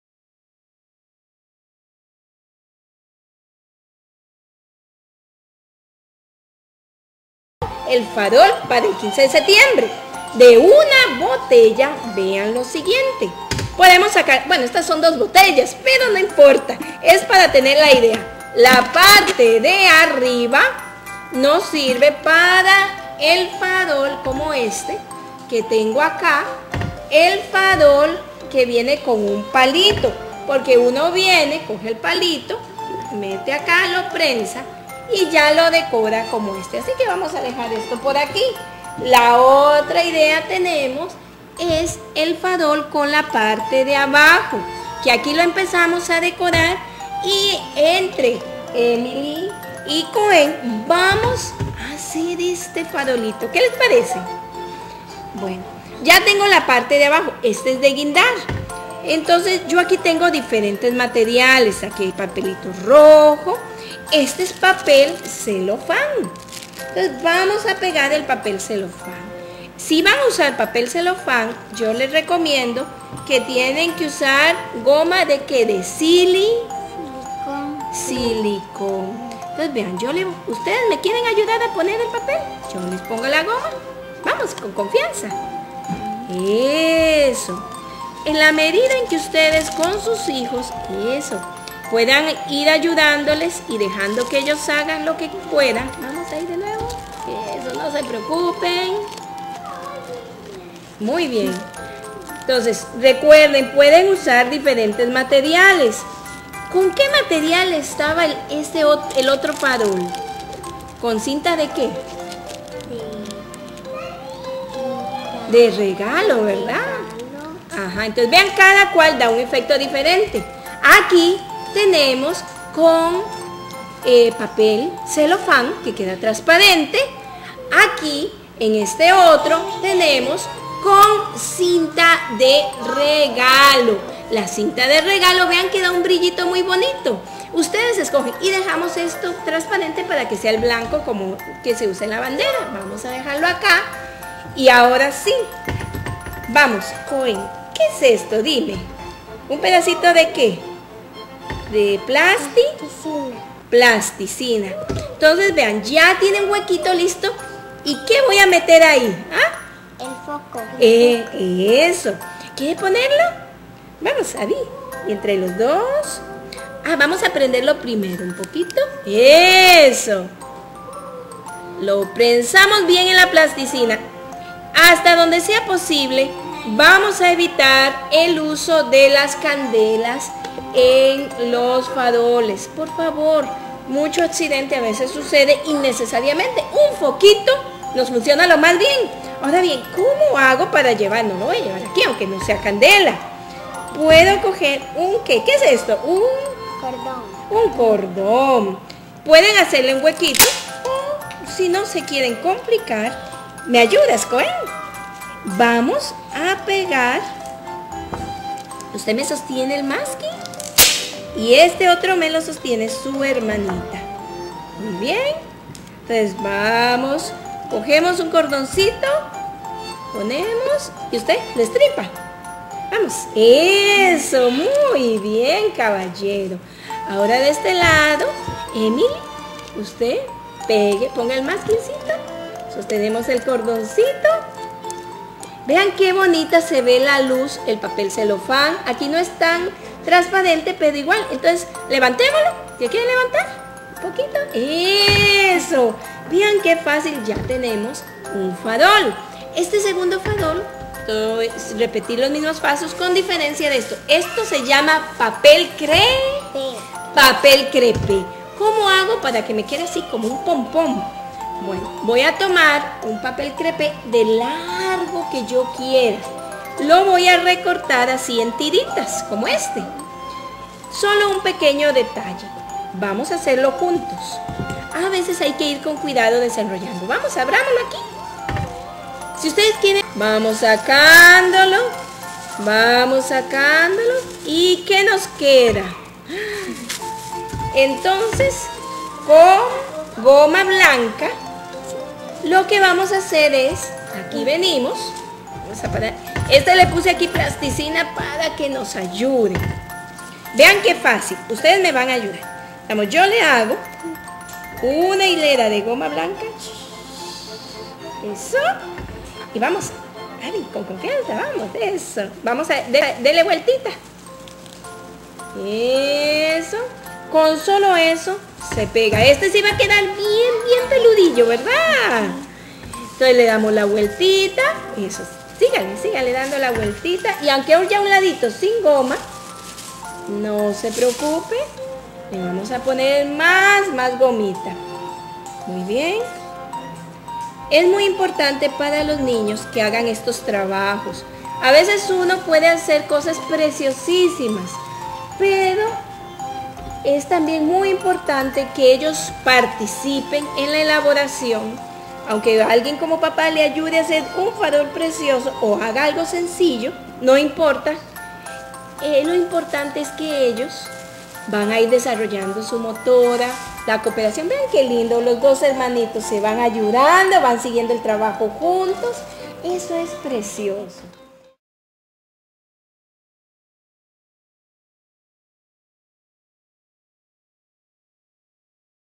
Farol de reciclaje en Cuenticos para el 14 y 15 de septiembre.
¡Hagamos un farol de reciclaje! Fácil,económico y divertido. ¡Faroles! Una manualidad especial para niños y niñas.  Y así celebrar fechas patrias.Pueden ver más ideas didácticas y manualidades a través de nuestro sitio en Facebook.
Dios les Bendiga.  Con cariño Ana María.